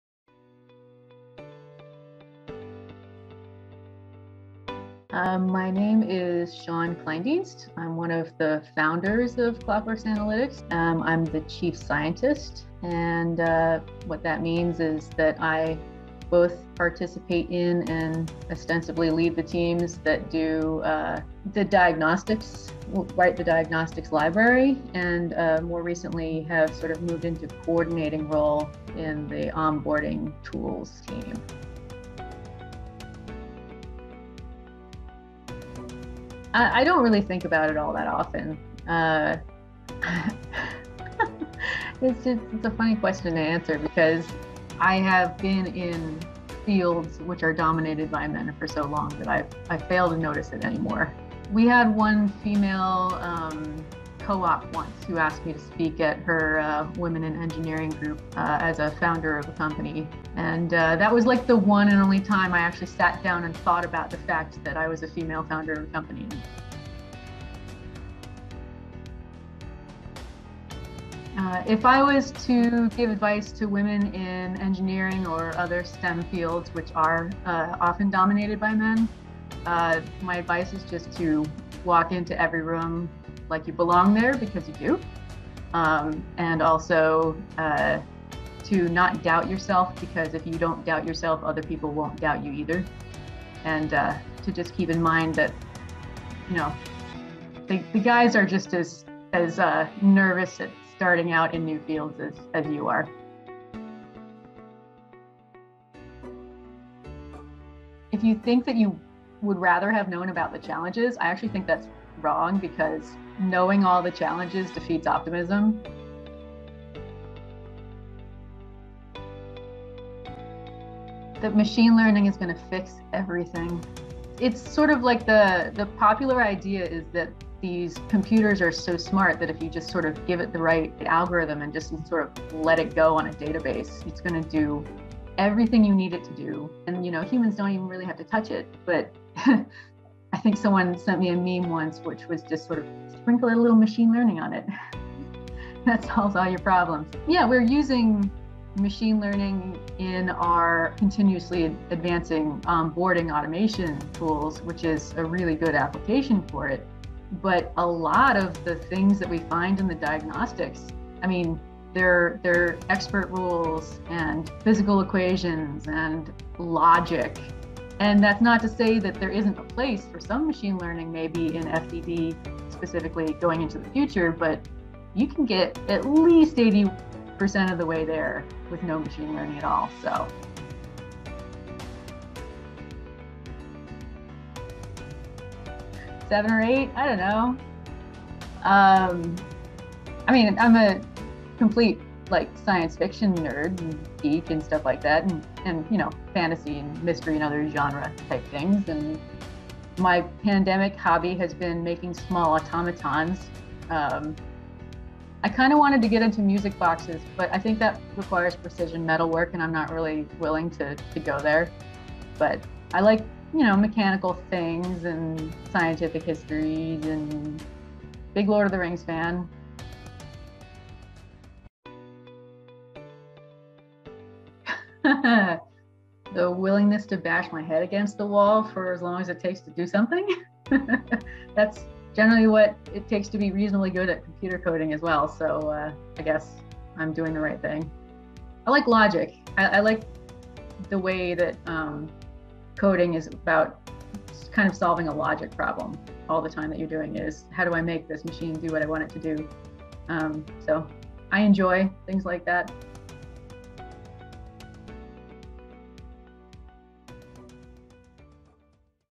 Siân Kleindienst, founder of Clockworks Analytics—5 in 5
Meet the female founder of Clockworks and hear her advice for other women pursuing careers in STEM, the FDD misconception that drives her crazy, her new pandemic hobby, and more!